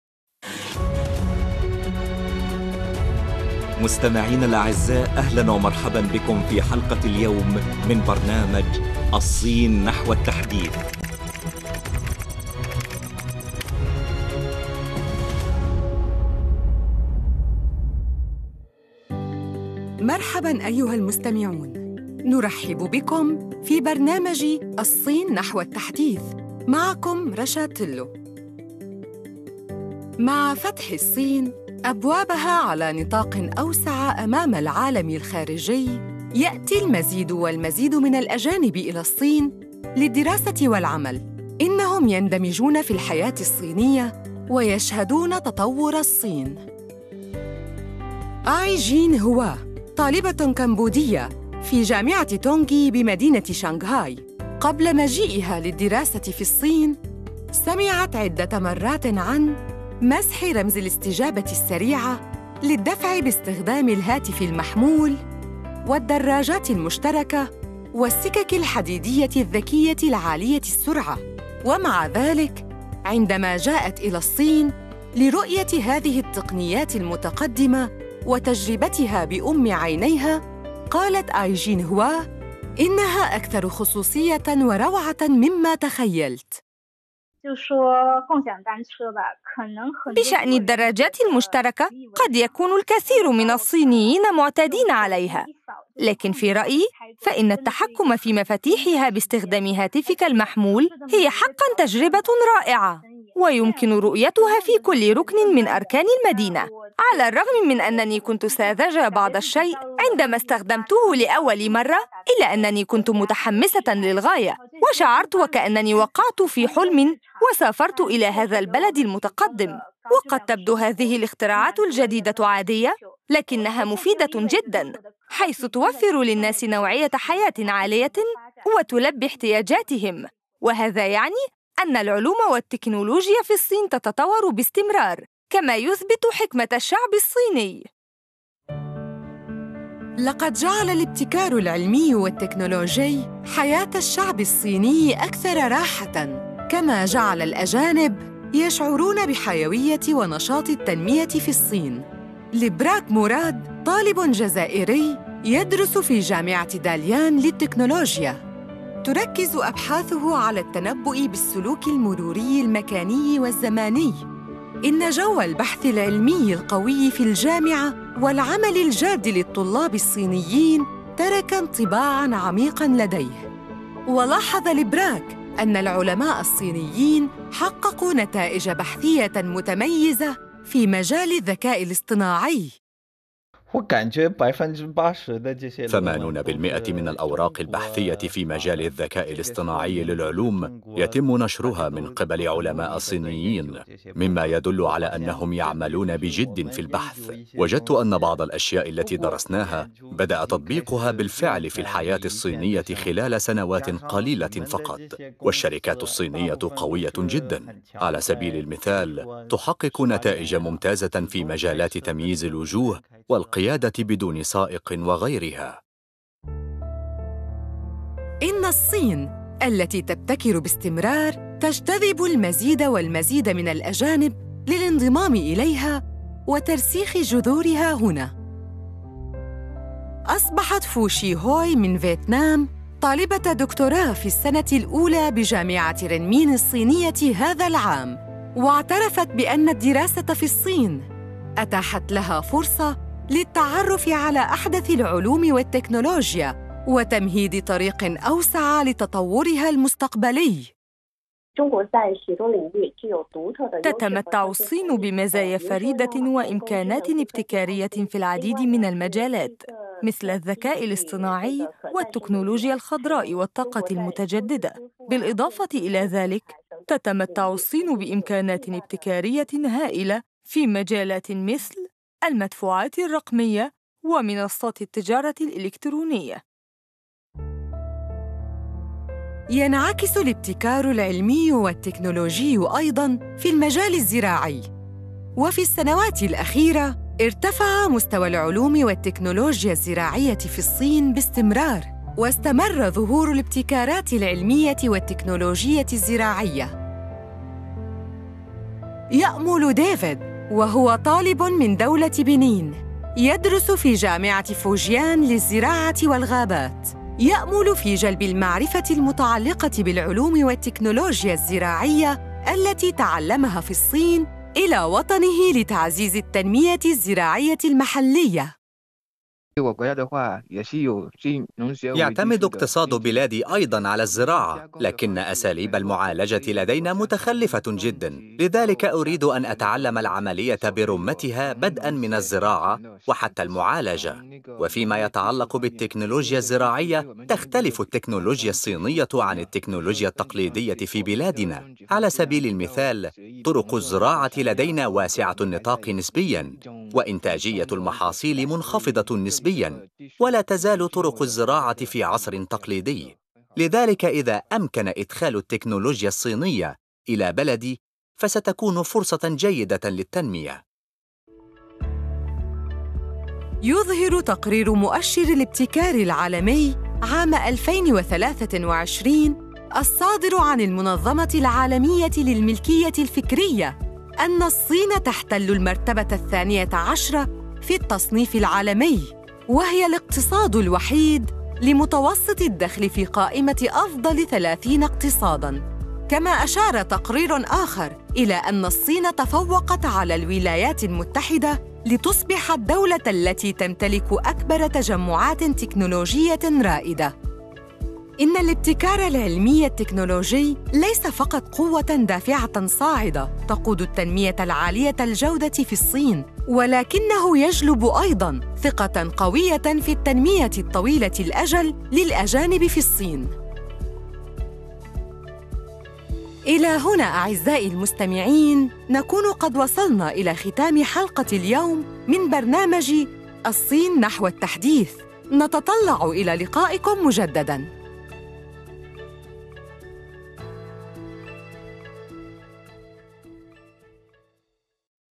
الابتكار التكنولوجي يعزز ثقة الأجانب في التنمية في الصين
آي جين هوا، طالبة كمبودية في جامعة تونغجي بمدينة شانغهاي، قبل مجيئها للدراسة في الصين، لقد سمعت عدة مرات عن " مسح رمز الاستجابة السريعة للدفع باستخدام الهاتف المحمول"، والدراجات التشاركية، والسكك الحديدية الذكية العالية السرعة. ومع ذلك، عندما جاءت إلى الصين لرؤية هذه التقنيات المتقدمة وتجربتها بأم عينيها، قالت آي جين هوا إنها أكثر تميزا وروعة مما تخيلت.